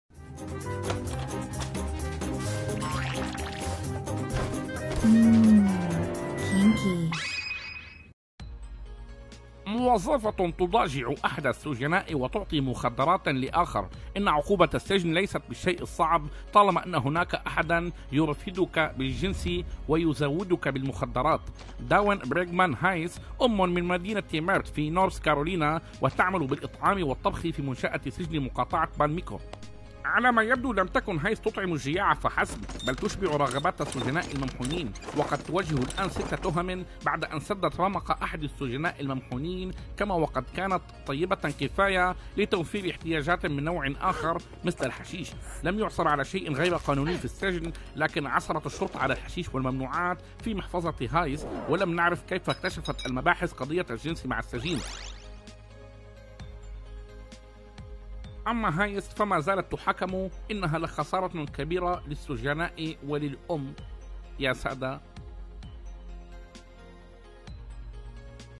أم سجانة تعطي النزلاء المخدرات والجنس في سجن مقاطعة بالميكو في نورث كارولينا
موظفة تضاجع أحد السجناء وتعطي مخدرات لآخر
إن عقوبة السجن ليست بالشيء الصعب طالما أن هناك أحدا يَرْفِدُكَ بالجنس ويزودك بالمخدرات.
داون بريغمان هاييس أمٌ من مدينة ميرِت في نورث كارولينا وتعمل بالإطعام والطبخ في منشأة سجن مقاطعة بالميكو.
على ما يبدو لم تكن هاييس تطعم الجياع فحسب, بل تشبعُ رغبات السجناء الممحونين
وتواجه الآن ستة تهم بعد أن سدت رمق أحد السجناء الممحونين... كما وقد كانت طيبة كفاية لتوفير احتياجات من نوع آخر.. مثل الحشيش
لم يعثر على شيء غير قانوني في السجن لكن عثرت الشرطة على الحشيش والممنوعات في محفظة هاييس
ولم نعرف كيف اكتشفت المباحث قضية الجنس مع السجين, أما هاييس فمازالت تُحاكم. إنها لخسارة كبيرة للسجناء وللأم.